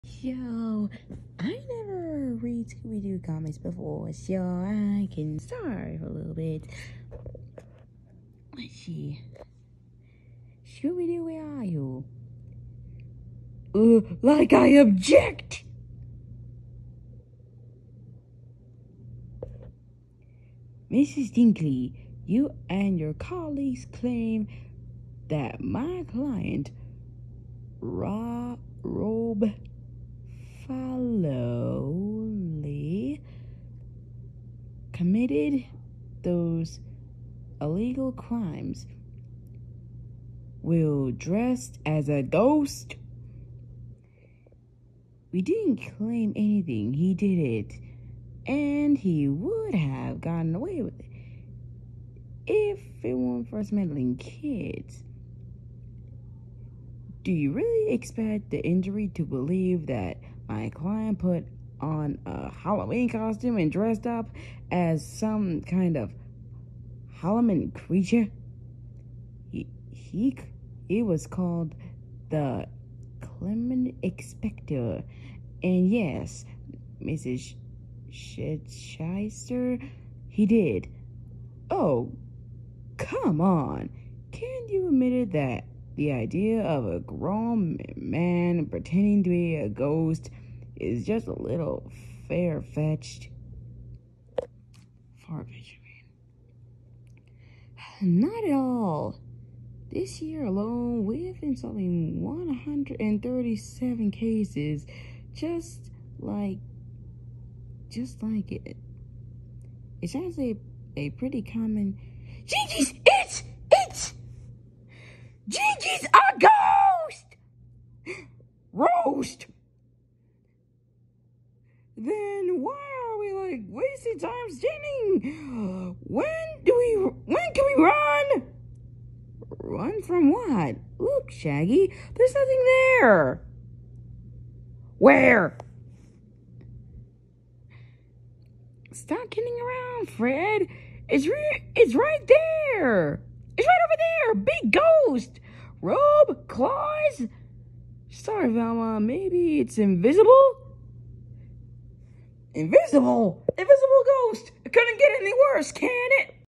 My first Scooby-Doo comic book read preview
I haven’t read Scooby-Doo books before so I try it I hope you’ll love it